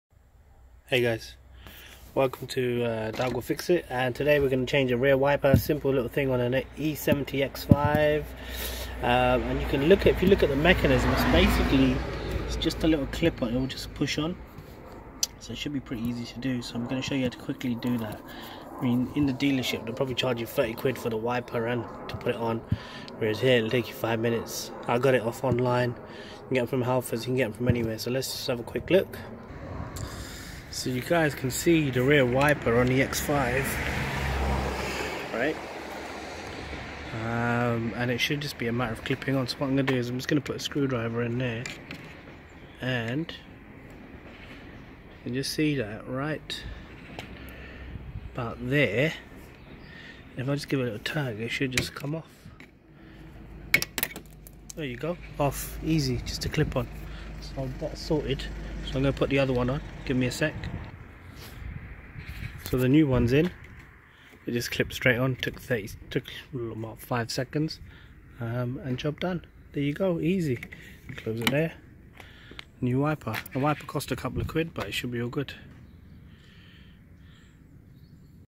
BMW Rear wiper Replacement change
Quick video showing how to change the rear wiper on an E70 X5. Pretty Simple